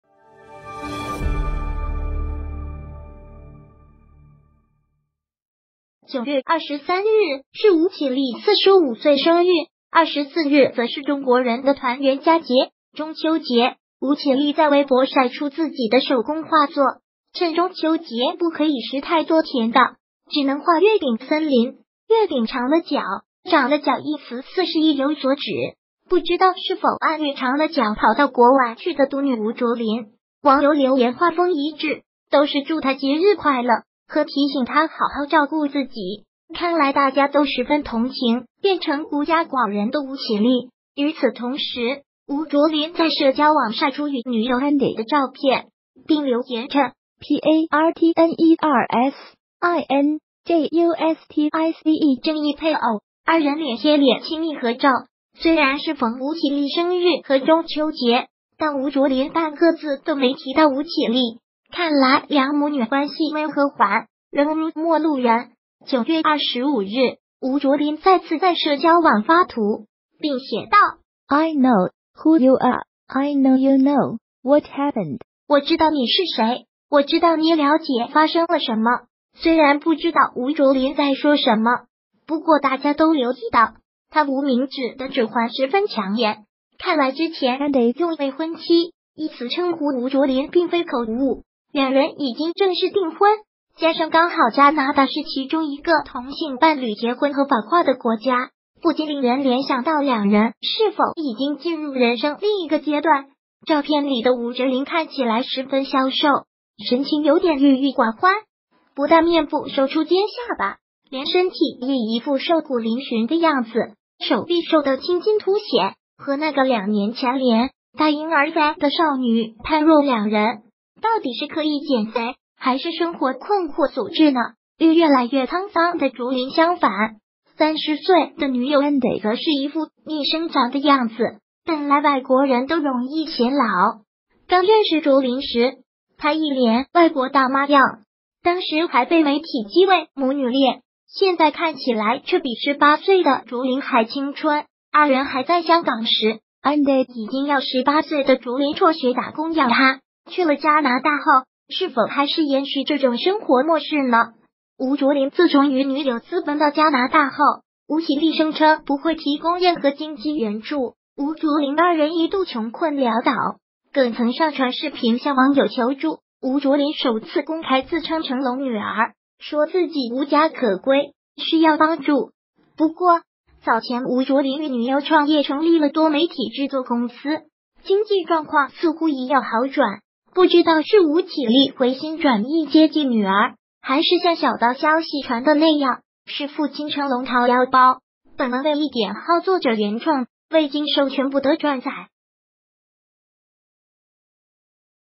疑似结婚？吴卓林最新照片瘦骨嶙峋，但30岁女友却逆生长
9月23日是吴绮莉45岁生日，24日则是中国人的团圆佳节——中秋节，吴绮莉在微博晒出自己的手工画作，称"中秋节不可以食太多甜的，只能画月饼森林。月饼长了脚..……”。“长了脚”一词似是意有所指，不知道是否暗喻“长了脚”跑到国外去的独女吴卓林。网友留言画风一致，都是祝她节日快乐和提醒她好好照顾自己，看来大家都十分同情变成“孤家寡人”的吴绮莉。与此同时，吴卓林在社交网晒出与女友Andi的照片，并留言称：“Partners.in.Justice（正义配偶）”，二人脸贴脸亲密合照。虽然适逢吴绮莉生日和中秋节，但吴卓林半个字都没提到吴绮莉，看来两母女关系未和缓，仍如陌路人。9月25日，吴卓林再次在社交网发图，并写道：“I know who you are. I know you know what happened.（我知道你是谁。我知道你了解发生了什么。）”虽然不知道吴卓林在说什么，不过大家都留意到，她无名指的指环十分抢眼，看来之前Andi用“未婚妻”一词称呼吴卓林并非口误，两人已经正式订婚。加上刚好加拿大是其中一个同性伴侣结婚合法化的国家，不禁令人联想到两人是否已经进入人生另一个阶段。照片里